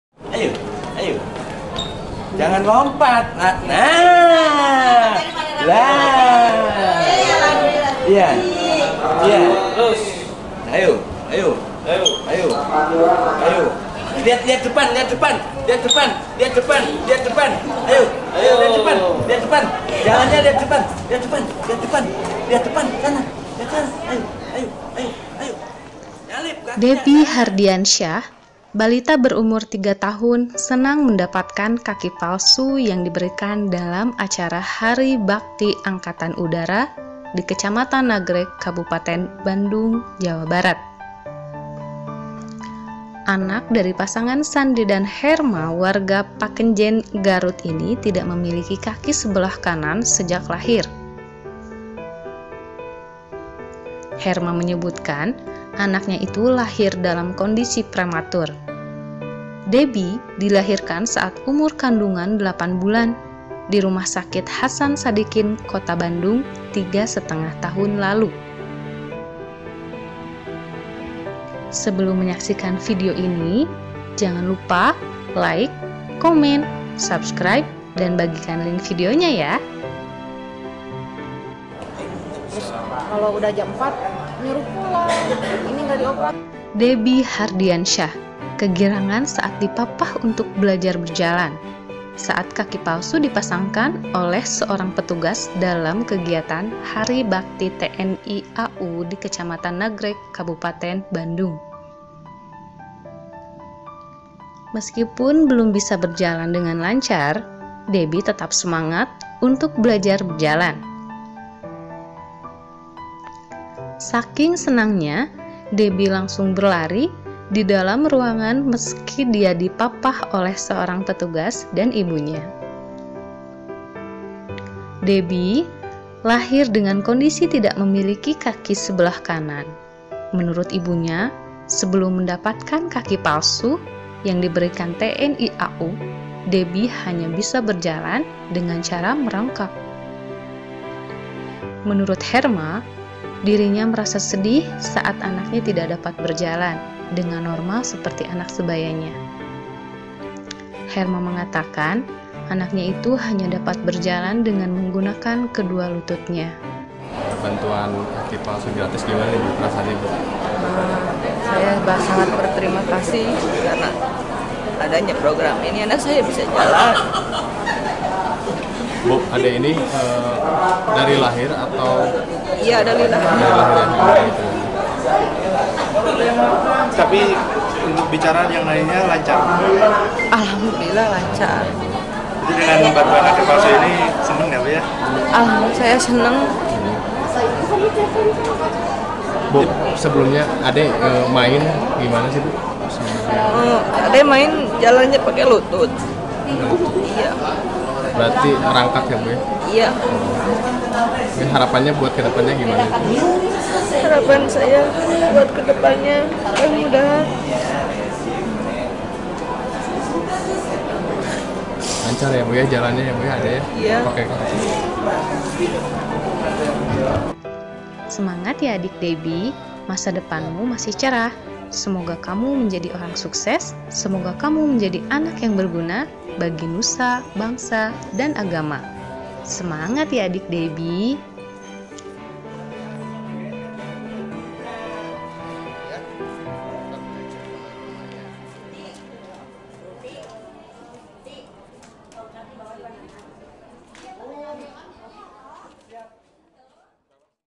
Kaki Palsu untuk Balita Deby Asal Garut
Deby Hardiansyah (3) anak dari pasangan Herma (25) dan Sandi (30) asal Pakenjeng, Kabupaten Garut senang mendapatkan kaki palsu yang diberikan TNI AU dalam acara Hari Bakti Angkatan Udara di Kecamatan Nagreg, Kabupaten Bandung, Jawa Barat.